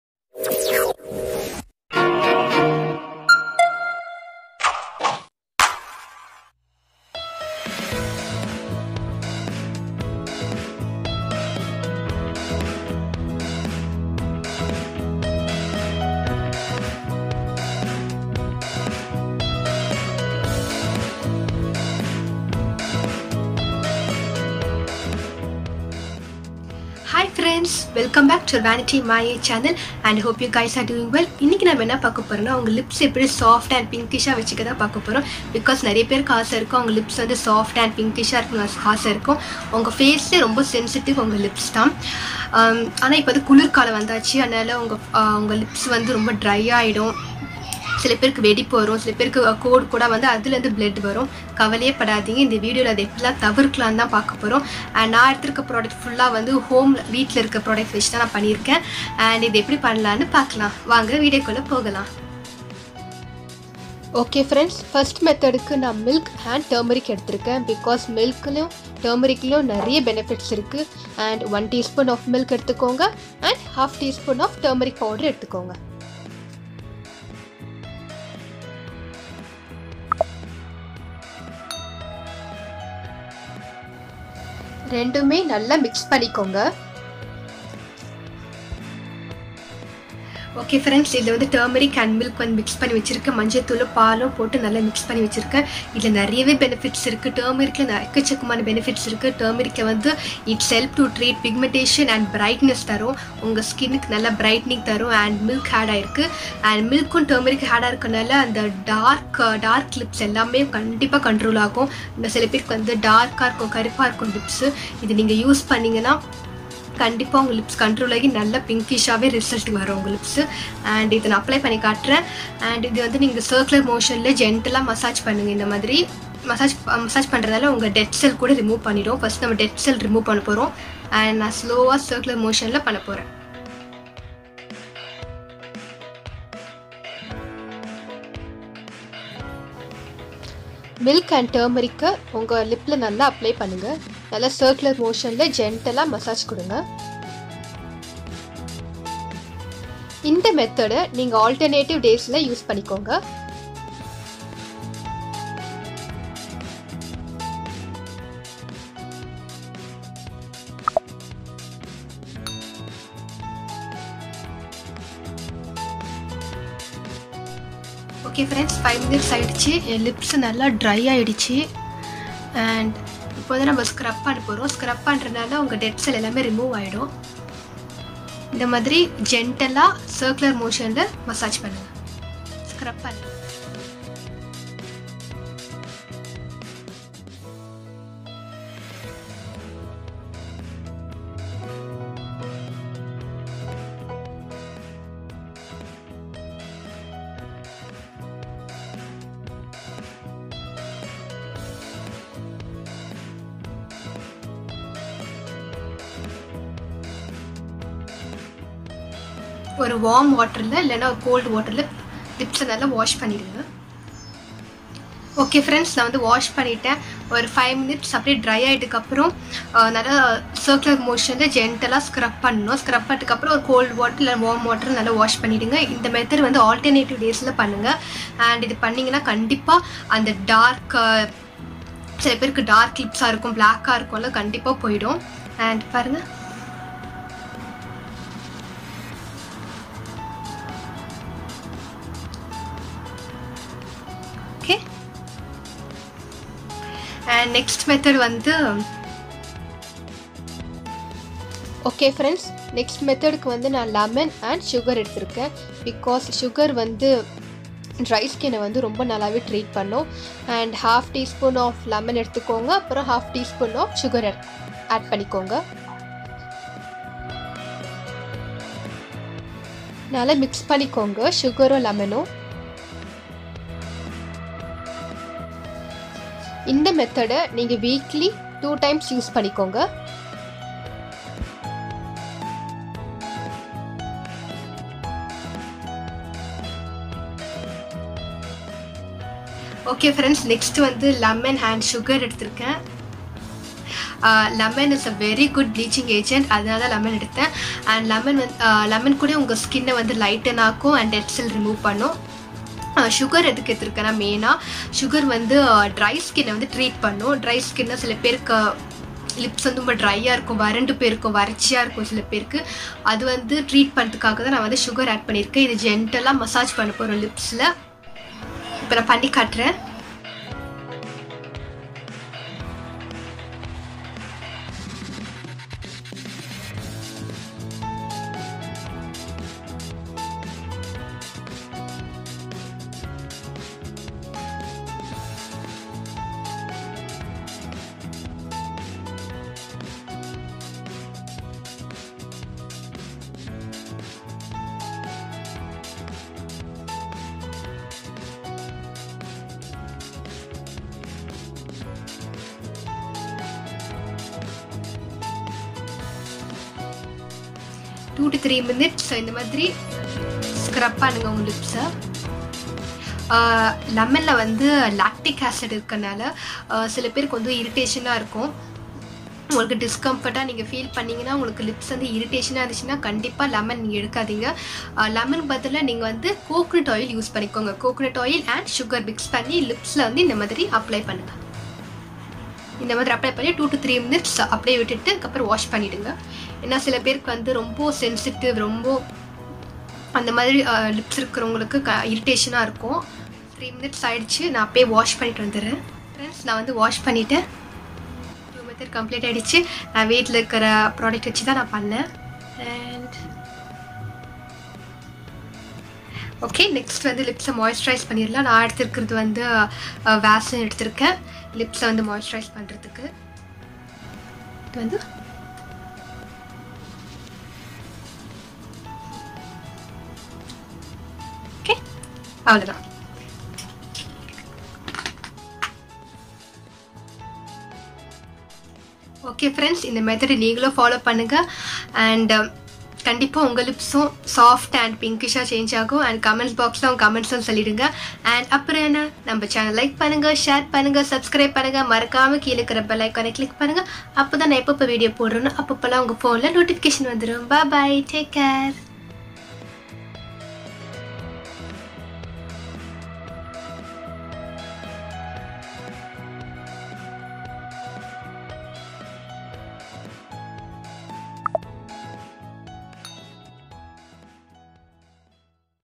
Permanent Soft & Pink lips Naturally at Home | Remove Dark Lips | உதடு பராமரிப்பு Tips | Rose Beauty
Today I am sharing how to get PINK LIPS 👄 Naturally by 2 easy steps.
Permanent Solution for Dark Lips/ Dry Lips / Home Remedy Tamil..

Ingredients Used :
 ▪️ Turmeric & Milk
 ▪️ Lemon & Sugar